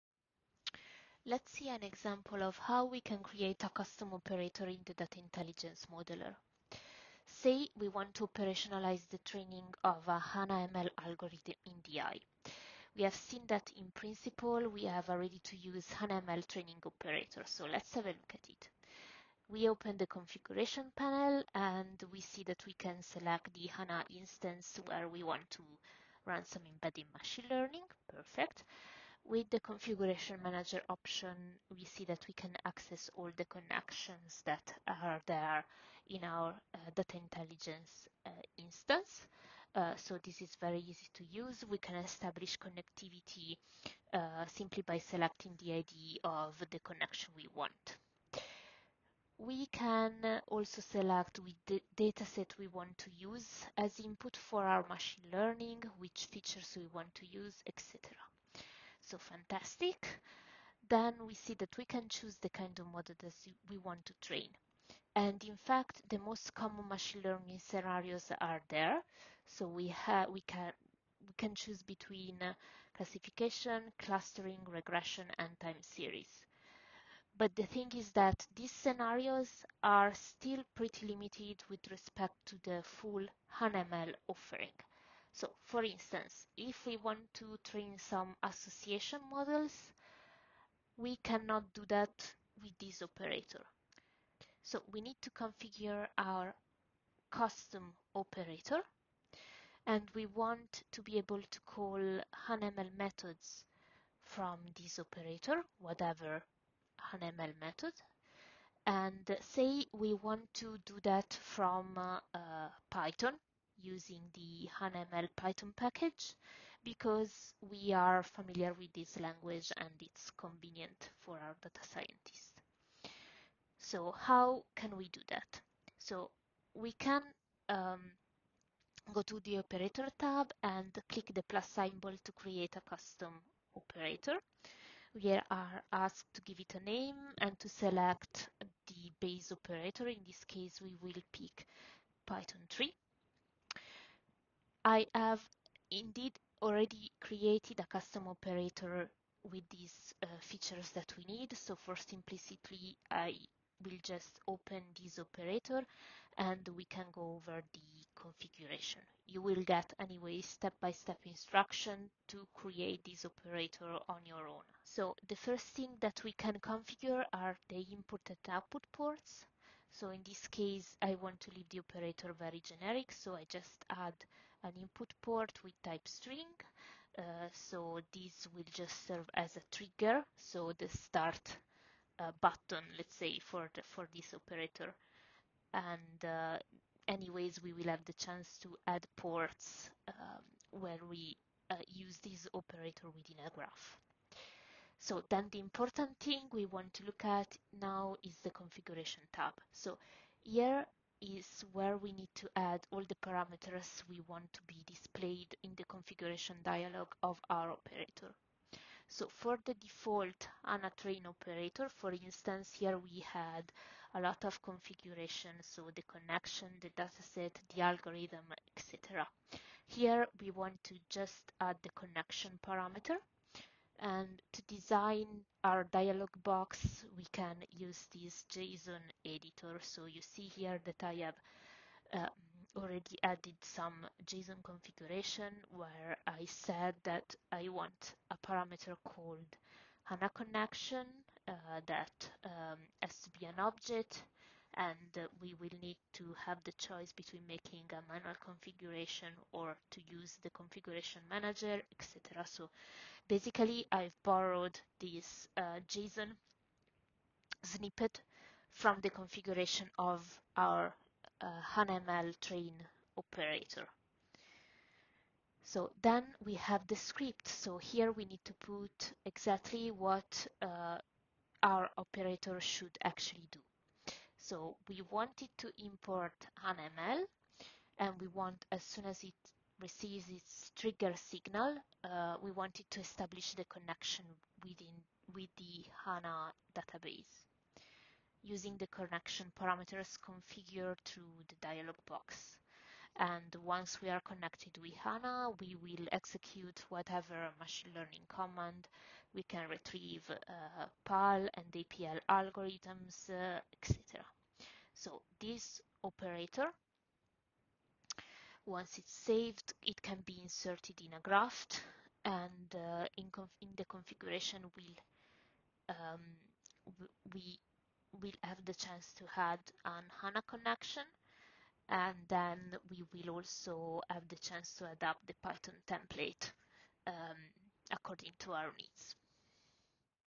DV230-Demo#1 Custom Operator in SAP Data Intelligence Cloud
This demo video will show you how to create a custom operator for HANA ML(PAL) Training based on Python operator about book recommendation with SAP Data Intelligence Cloud, as part of SAP Data-to-Value Bootcamp series